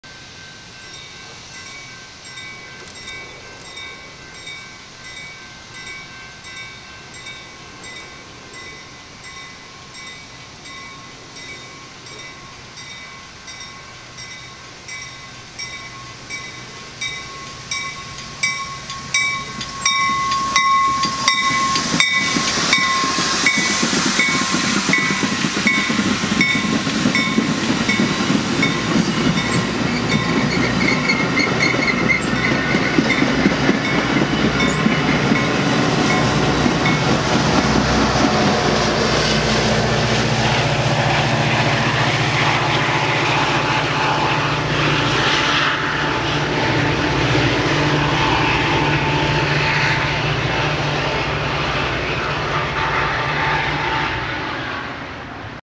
Metra 8577 East in Berwyn, IL 7/31/23
EB Metra, Harlem Ave., Berwyn, IL, BNSF Chicago Sub
Metra 8577 = Nippon Sharyo Cabcar
Metra 181 = F40PH-3